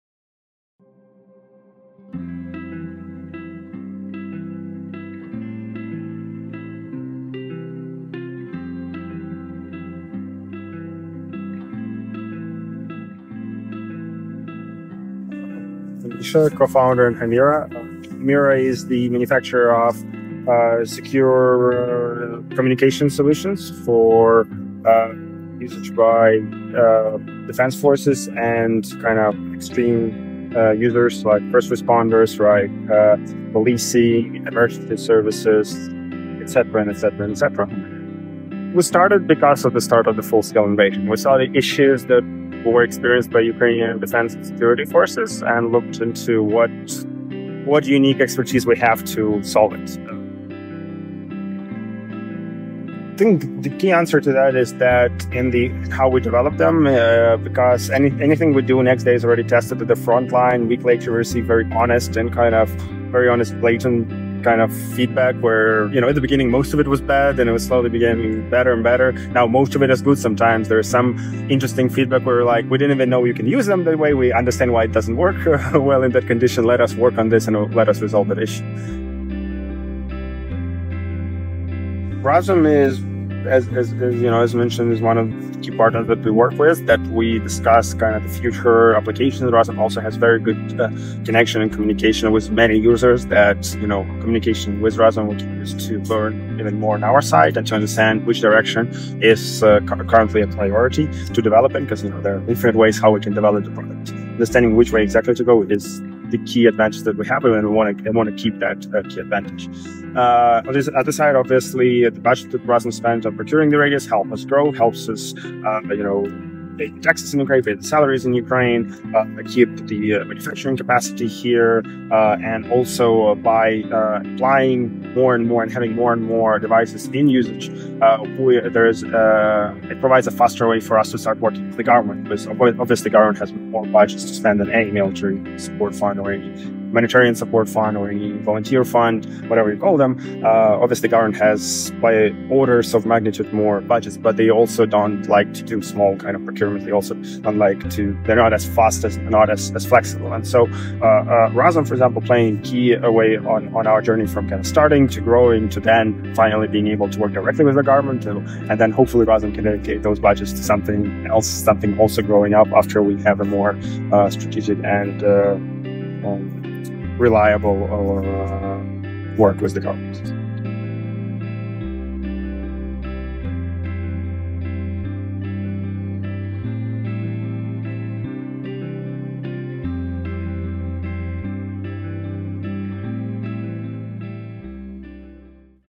Innovating for the Future: Razom for Ukraine and Himera Radios 🇺🇦 Misha Rudominski
Razom proudly procures and distributes life-saving Himera radios, cutting-edge communication technology developed by Ukrainian engineers to ensure reliable communication in emergency settings.

By purchasing Himera radios, Razom is driving innovation that makes a real difference. Our initial order of more than 2100 radios was crucial in launching this life-saving technology, which is now making waves in international markets. Himera radios are designed for both civilian and emergency use, enhancing communication for frontline first responders and bolstering Ukraine’s economy.

Recently, Himera presented their latest G1 Pro L radios, and our Razom team had the opportunity to test them. After testing these radios, we spoke with Misha Rudominski, Himera’s co-founder, about their development and our partnership.

📹 Watch our interview with Misha Rudominski to learn more about Himera’s development and work with Razom.

Explore more about Himera at himeratech.com.
To support Razom’s efforts and help Ukraine, please donate at razomforukraine.org.